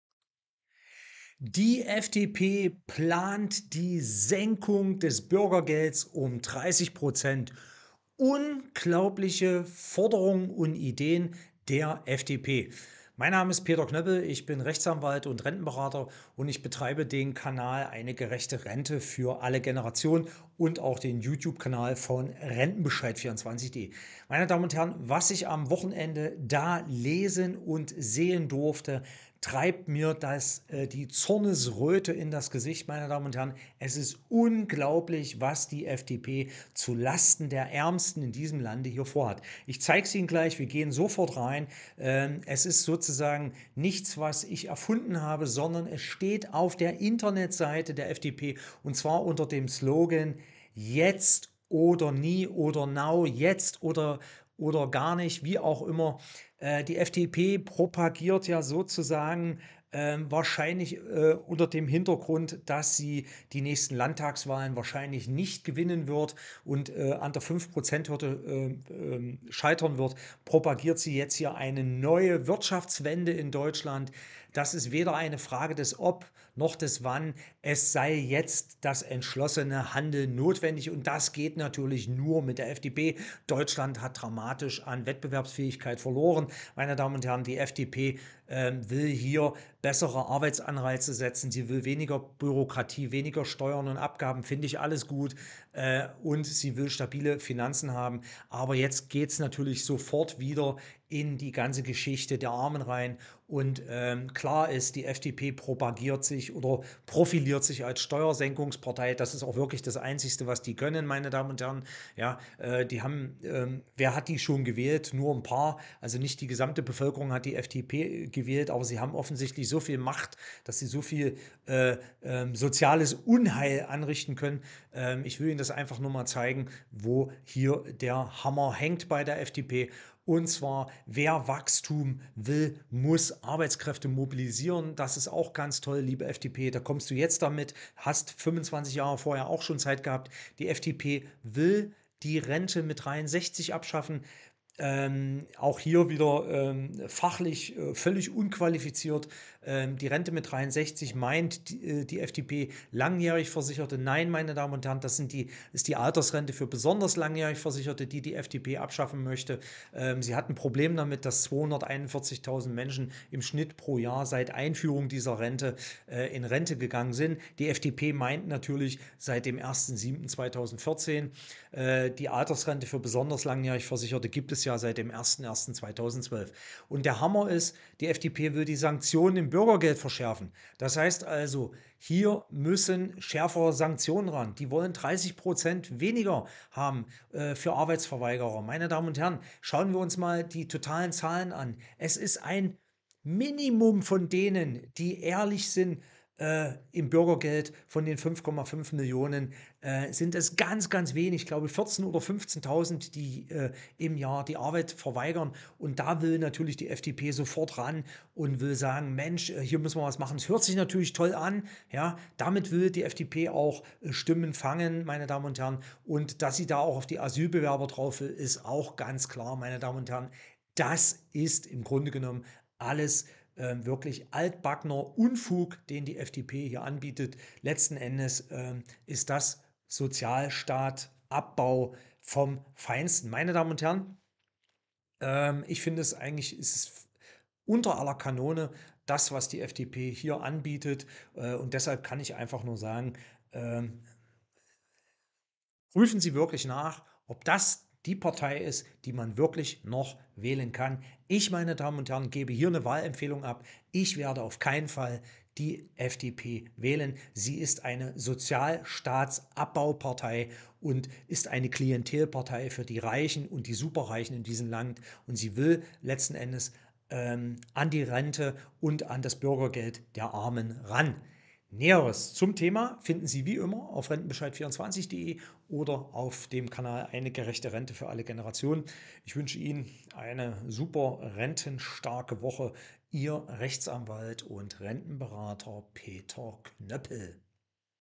FDP bläst zum Generalangriff auf die Rente: Abschaffung Rente mit 63, Rentensenkung/ Bürgergeld
Jetzt oder Nie heißt das neue Wirtschafts"_Wunderprogramm" der FDP. Nachzulesen auf deren Internetseite. Die FDP plant unter anderem die Abschaffung der Rente mit 63, die Einführung einer flexiblen Altersrente nach schwedischem Modell unter Inkaufnahme von hohen Abschlägen und Rentenkürzungen und die Streichung von bis zu 30 Prozent Bürgergeld bei Arbeitsverweigerern.
Ist das der Totalangriff auf den Sozialstaat? In diesem Video zeigen wir um was es in der Sache geht.